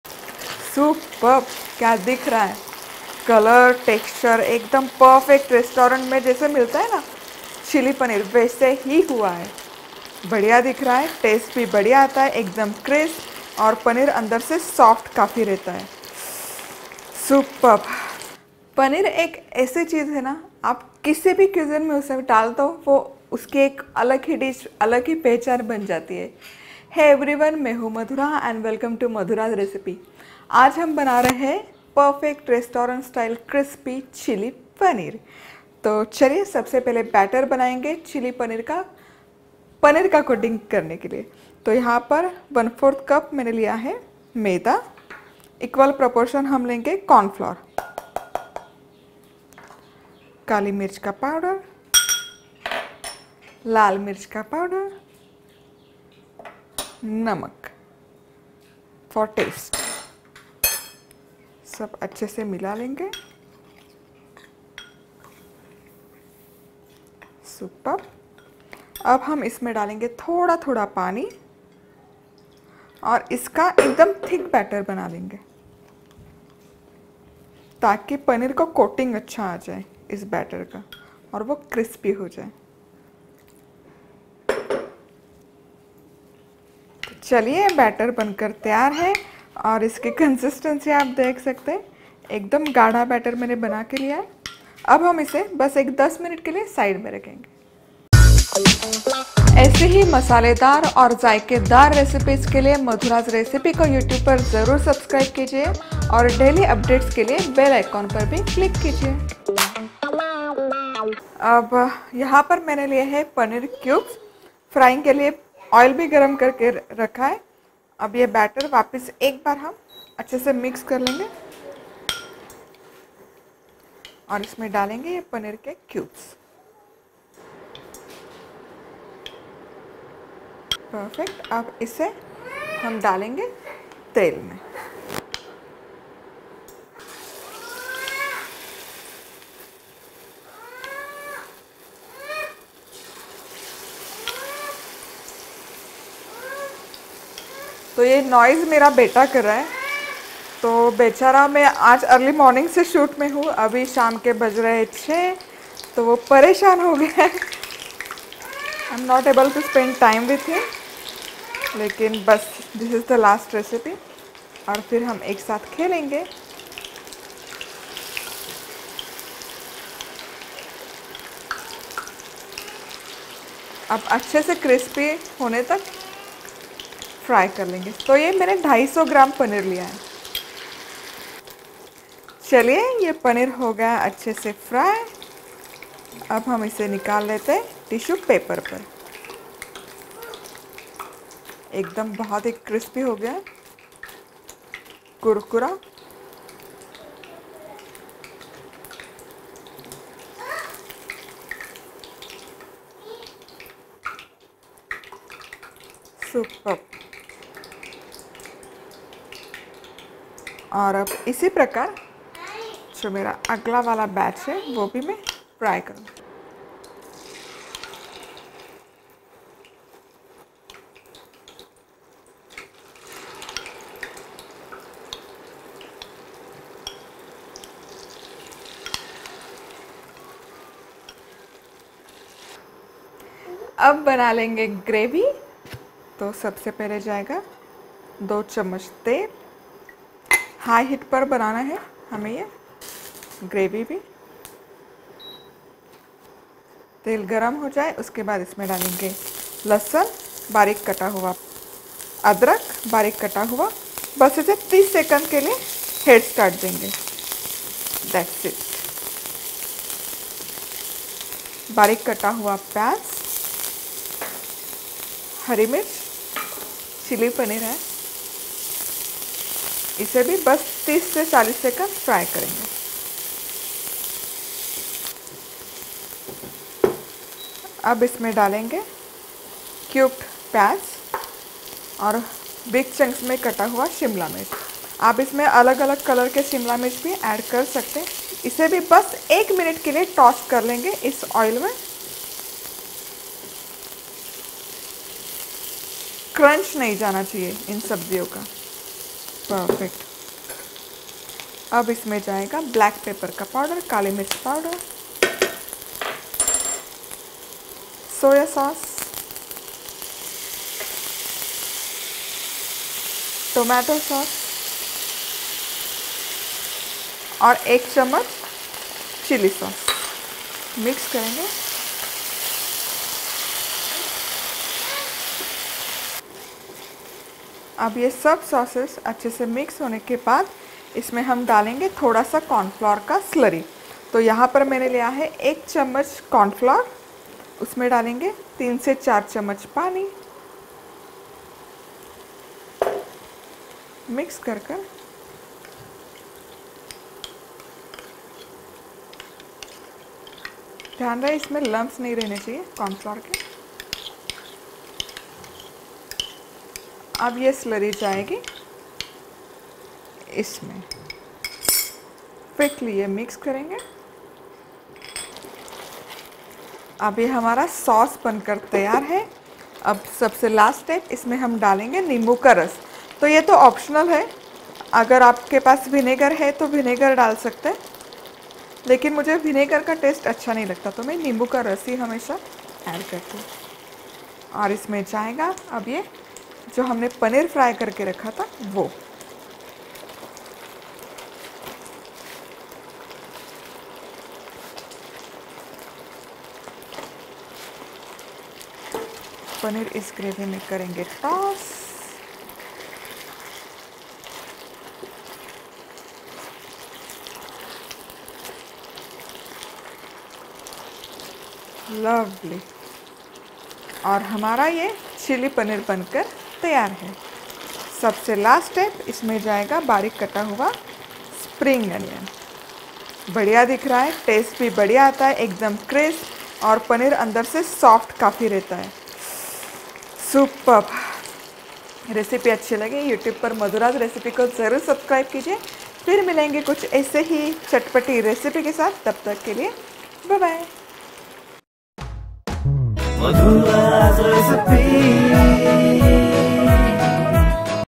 रेस्टॉरंट स्टाईल चिली पनीर बनाने का तरीका | Restaurant Style Chilli Paneer Recipe | MadhurasRecipe
Chili paneer is a popular Indo Chinese recipe. Paneer cubes are coated in batter and fried. These are then cooked in spicy Chinese sauce. It goes well with noodles or fried rice. This can be a good starter option. You can make it dry, semi dry or in gravy. You can try this recipe at home and drop a comment for me.

Ingredients:
• 1/4 cup Maida
• 1/4 cup Corn flour
• 1/4 tsp Black pepper powder
• 1/2 tsp Red chilli powder
• Salt to taste
• Water
• 250 grams Paneer cubes
• Oil for frying
• 2 tsp Oil
• 1 tbsp finely chopped Garlic
• 1 tbsp finely chopped Ginger
• 1 tbsp finely chopped Onion
• Green chilies
• 2 tbsp Onion cubes
• 2 tbsp Capsicum chopped into big chunks
• 1 tsp Black pepper powder
• 1 tsp Soya sauce
• 1 tsp Tomato sauce
• 1 tsp Red chili sauce
• 1 tbsp Corn flour
• 3~4 tsp Water
• Lemon juice
• Finely chopped Spring onion

Method:
• Take maida in a bowl and add corn flour, black pepper powder, red chilli powder, salt.
• Mix everything well together and add a little water at a time to make thick batter.
• Paneer gets good coating due to thick batter.
• Rest the batter for about 10 minutes.
• Heat up oil in a pan.
• Mix the batter well once and drop the paneer cubes into it.
• Coat the paneer cubes well with batter and drop them into hot oil.
• Drop the cubes into hot oil and fry well until nice and crispy from both sides.
• When paneer is fried well take them out, drain excess oil and transfer on a tissue paper.
• Heat up oil on high heat in a pan.
• When oil is hot enough add garlic, ginger and fry for just 30 seconds.
• Add onion, green chilies and fry for just 30-40 seconds.
• Add onion cubes, capsicum and fry for just 1 minute. Veggies should not lose their crunch.
• You can use different colored capsicum if you want.
• Add black pepper powder, soya sauce, tomato sauce, red chili sauce and mix well.
• Take corn flour in a bowl and add water.
• Mix well to make slurry. No lumps of corn flour should be formed.
• Add this slurry into the veggies and mix quickly.
• Sauce is ready.
• Add lemon juice and mix well.
• You can use vinegar in place of lemon juice.
• Add fried paneer and mix well.
• Chili paneer is all ready.
• Sprinkle spring onion.

How to make French Fries at home | Secret Tip to keep fries Crisp | Homemade Crispy French Fries |